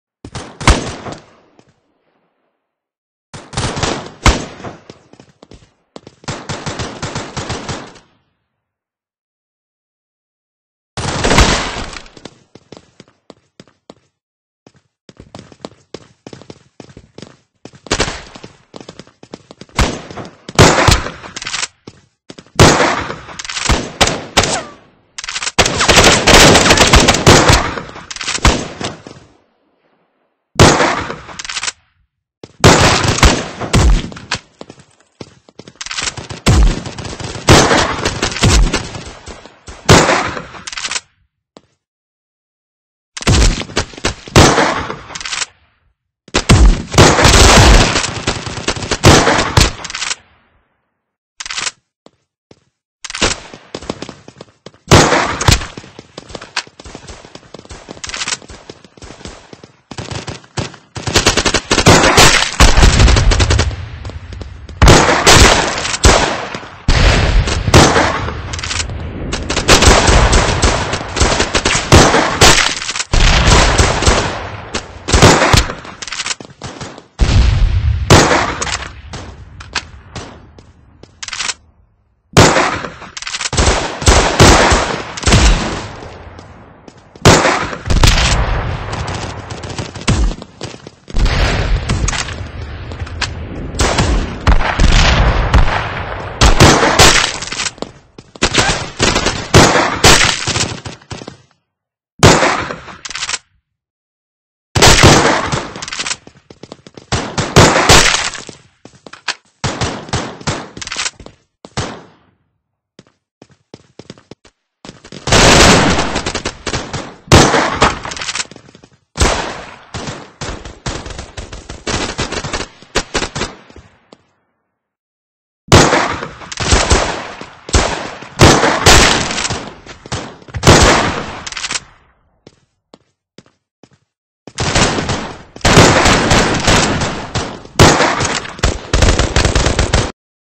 M40A1 Gun Testing
This weapon can be used via the Gun Master. You need 5000 NPC kills to unlock this.